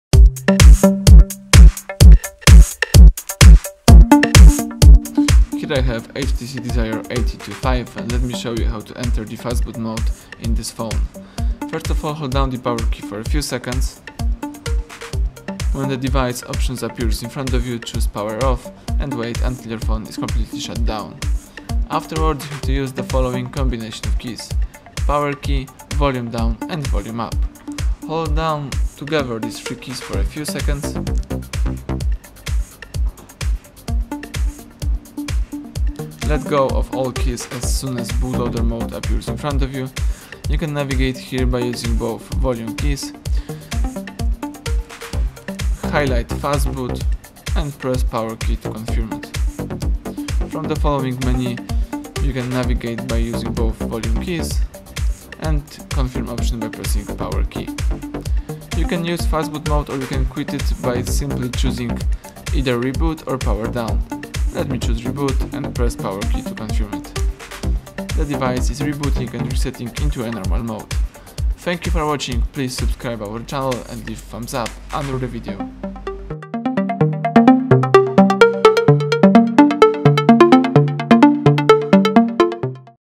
Fastboot Mode HTC Desire 825 - How to Enter Fastboot in HTC Desire
How to enter the fasboot mode? How to exit the fastboot? How to quit into normal mode? How to exit Fastboot Mode in  HTC Desire 825?
Fastboot is a protocol which may be used for updating the flash file system in your device. Using the fastboot you can modify the file system image from a comuter via USB connection. It may come in handy when you want to do a quick update of the firmware without having to use the recovery mode.